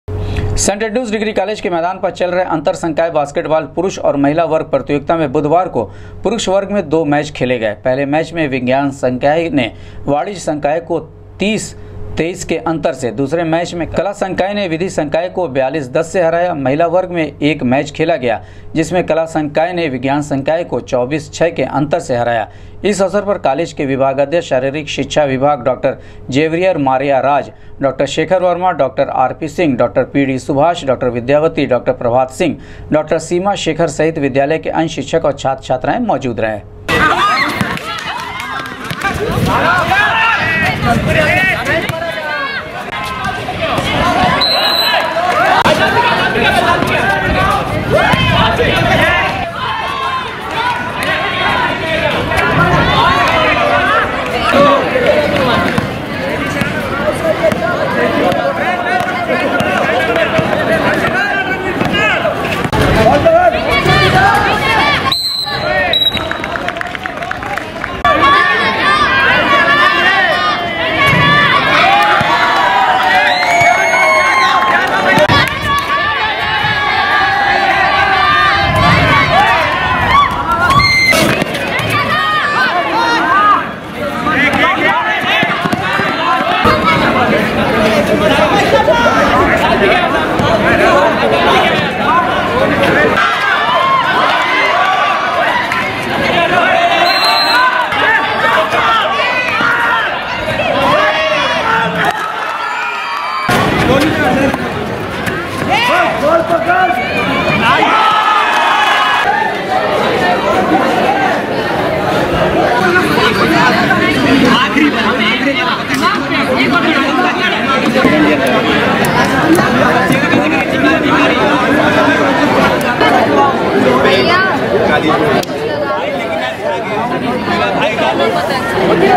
सेंट एंड्रयूज डिग्री कॉलेज के अंतर संकाय बास्केटबॉल प्रतियोगिता
सेंट एंड्रयूज डिग्री कॉलेज के अंतर संकाय बास्केटबॉल प्रतियोगिता में पुरुष  वर्ग में विज्ञान संकाय ने वाणिज्य संकाय, कला संकाय ने विधि संकाय को, और महिला वर्ग में कला संकाय ने विज्ञान संकाय को हराया